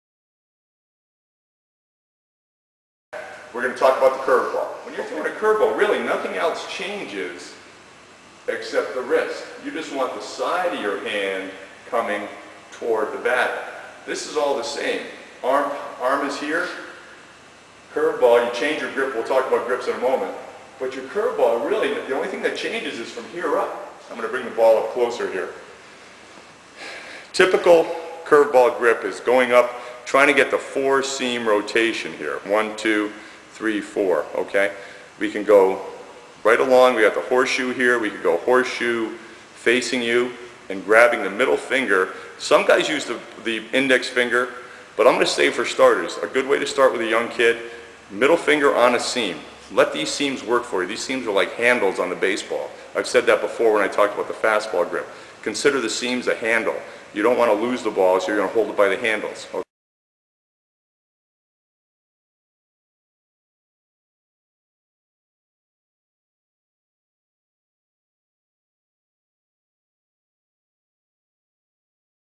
Throwing a Curve Ball - Dennis Long
If you're going to throw a curve ball, you better learn to do it right. Pitching coach Dennis Long reveals the secrets to throwing a great curve ball. For more info on Dennis and his pitching tips today.